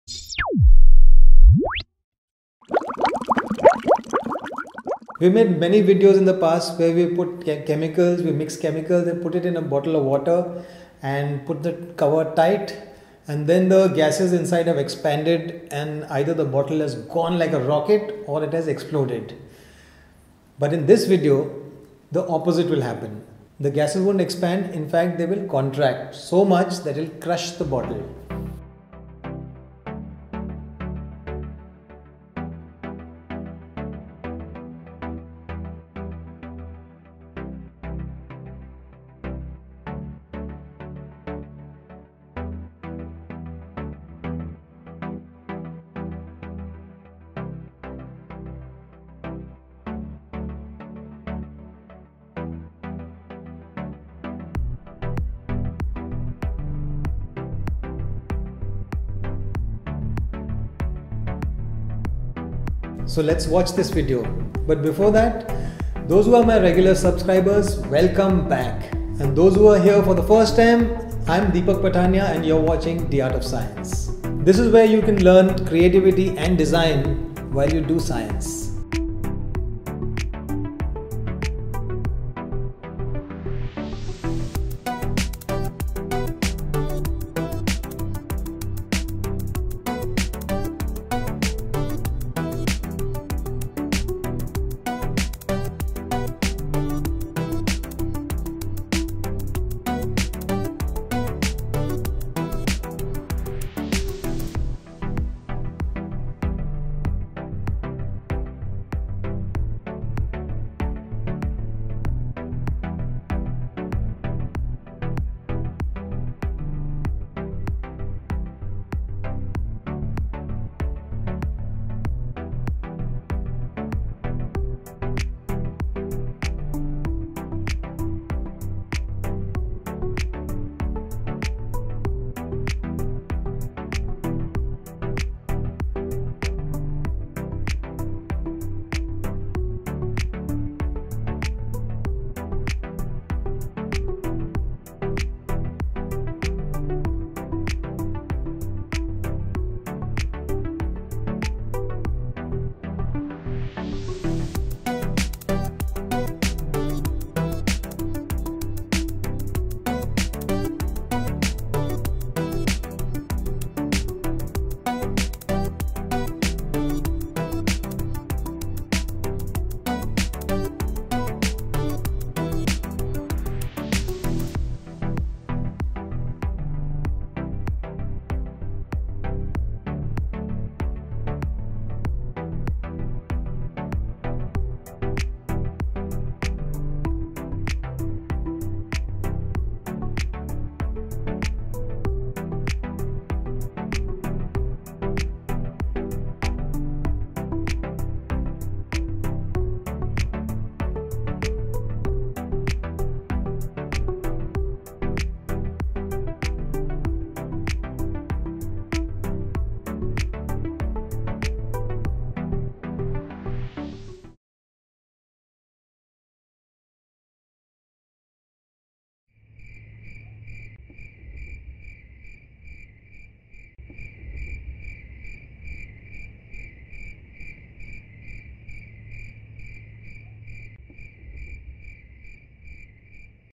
Chemical Compression DIY | dArtofScience
Howsey dowsey! This video shows a chemical compression DIY project that you could do at home but with adult supervision. Its fun. And after doing this and putting it here on YT, i realised there is something more that can be done with this, and i will soon make another video for this to show how one can make it work for you.